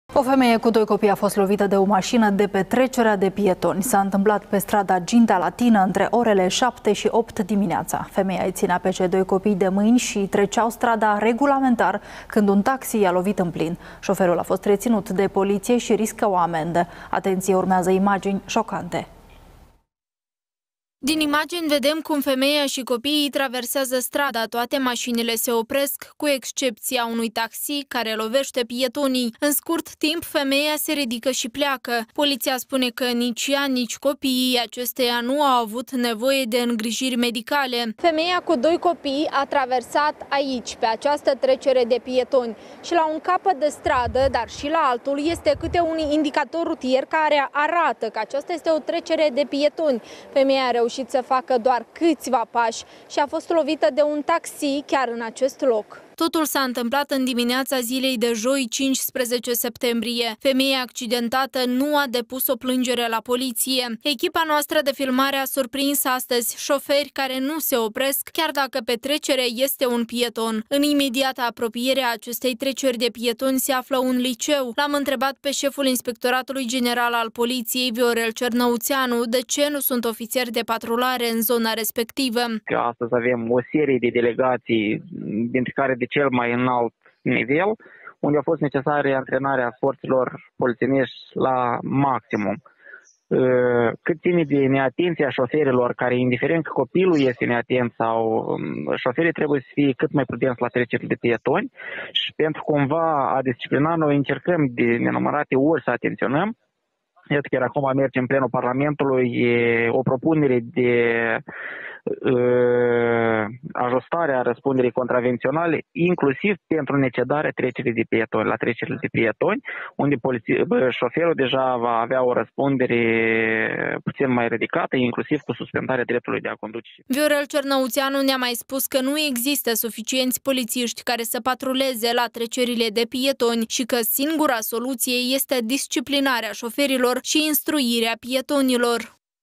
La un pas de tragedie
O femeie cu doi copii a fost lovită de o mașină, de pe trecerea de pietoni. S-a întâmplat pe strada Ginta Latină, între orele 7 și 8 dimineața. Femeia îi ținea pe cei doi copii de mâini și treceau strada regulamentar, când un taxi i-a lovit în plin. Șoferul este căutat de poliție. Atenție, urmează imagini șocante!